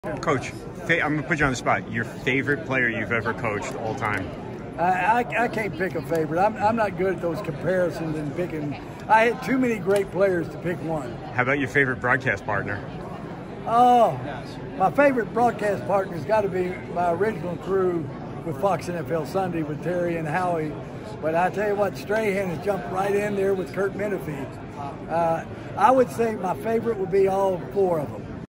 Jimmy Johnson Interview / The Bleacher Bros / Super Bowl 57/
AJ asks coach Jimmy Johnson the hard questions at the Fox Sports Super Bowl Press Conference in Phoenix, Az at Super Bowl 57